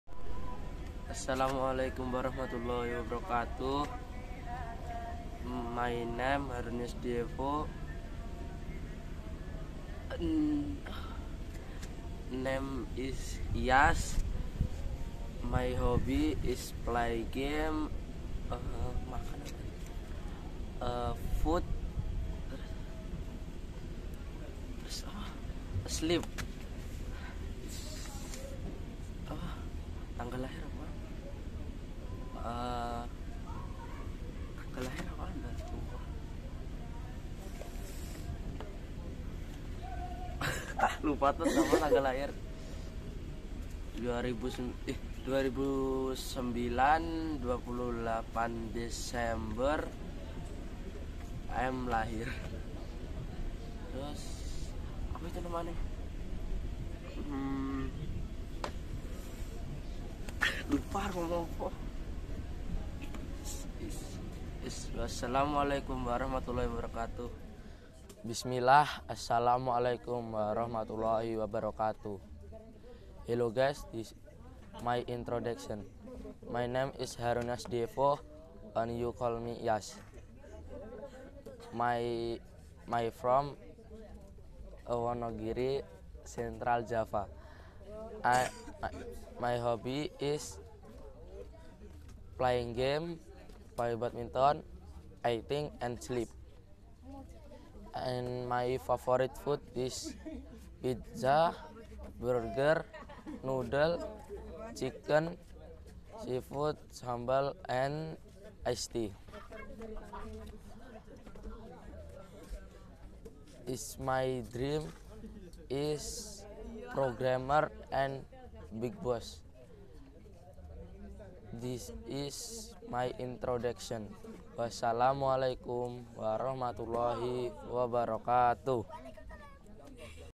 Haruniyas Diefo - English Introductory Program (Before-After)
Hi fellas! Here's our English introduction comparison video from the first time we joined IDN Introductory Program.